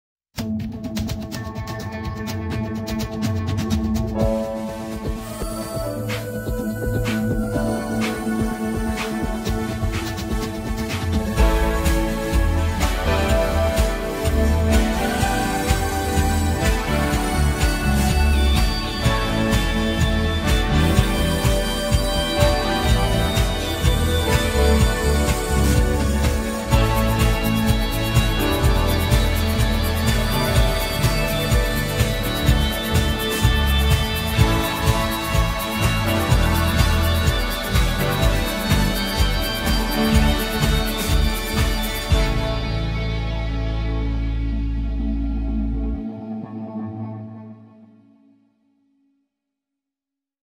Print Cleaning, Tape Removal
A fun time-lapse video of cleaning a print that had been taped down on all sides.  After some soaking in distilled water and some light brushing with a paintbrush, the tape came right off!